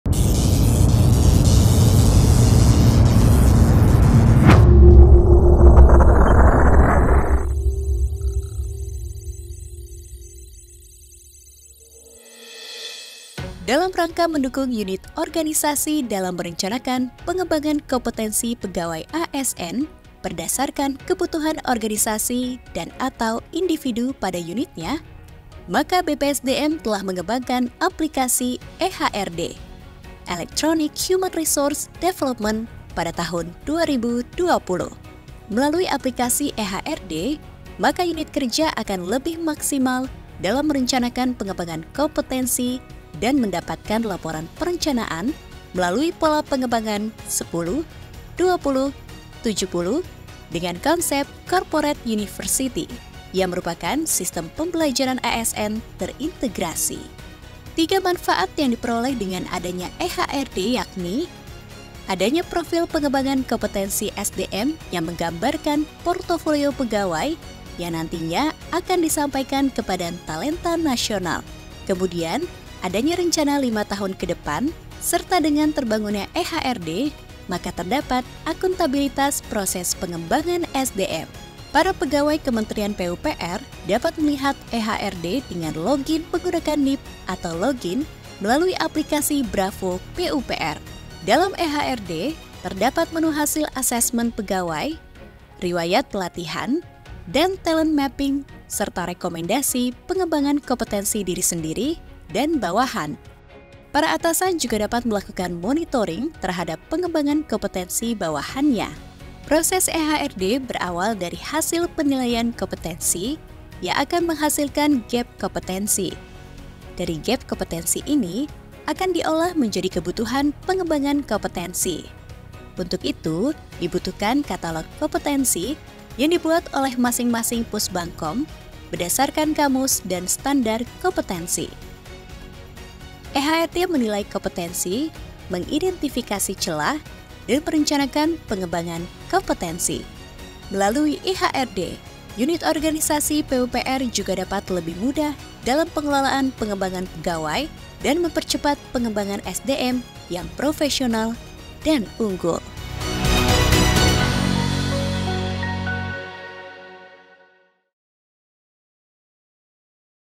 eHRD Kementerian PUPR
BPSDM telah mengembangkan aplikasi eHRD (Human Resource Development) pada tahun 2020, Melalui aplikasi eHRD maka unit kerja akan lebih maksimal dalam merencanakan pengembangan kompetensi di lingkungan BPSDM dan mendapatkan laporan perencanaan melalui pola pengembangan 10:20:70. dengan konsep Corporate University.